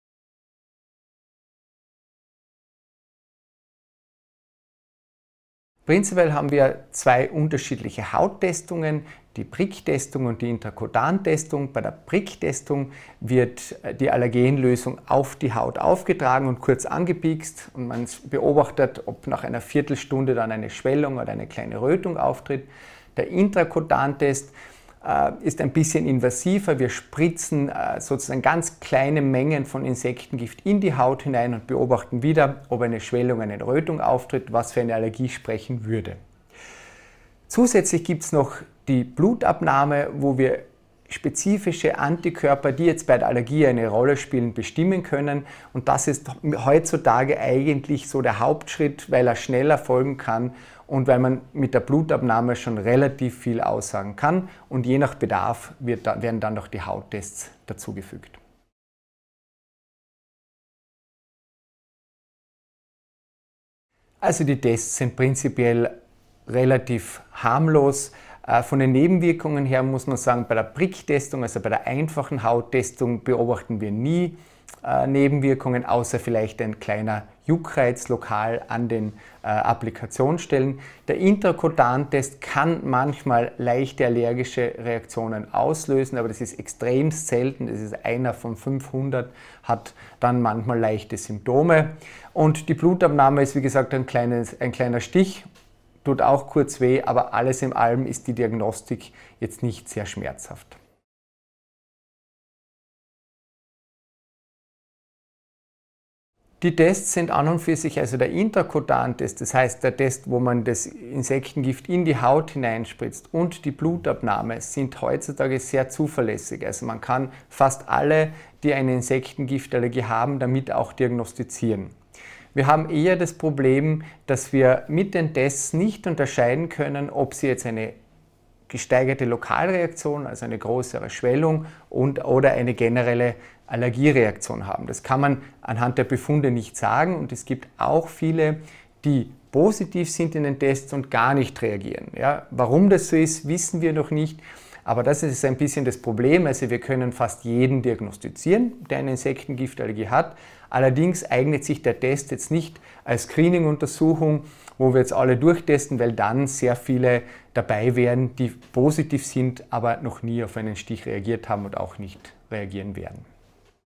Pricktest, Intrakutantest und Bluttest - so wird Insektengiftallergie festgestellt (Arzt erklärt)
Verdacht auf Insektegiftnallergie gestellt wird, sind zur Abklärung weitere Tests nötig. Was wird beim Allergietest getestet und wie unterscheiden sich Pricktest, Intrakutantest und Bluttest?

Assoz.-Prof. Dr. Gunter Sturm, Facharzt für Dermatologie und Venerologie, beantwortet im Video "Pricktest, Intrakutantest und Bluttest - so wird Insektengiftallergie festgestellt " folgende Fragen:

00:00 ► Welche Testmöglichkeiten gibt es bei Verdacht auf Insektengiftallergie? (Pricktest, Intrakutantest und Bluttest)
00:58 ► Kann es bei den Tests zu Schmerzen kommen und welche Nebenwirkungen sind möglich?
01:42 ► Was sagen die Ergebnisse der Tests aus?

✅ Was tun nach der Diagnose?

➡️ Hol Dir den kostenlosen selpers-Newsletter mit vielen Tipps und kostenlosen Downloads rund um deine Erkrankung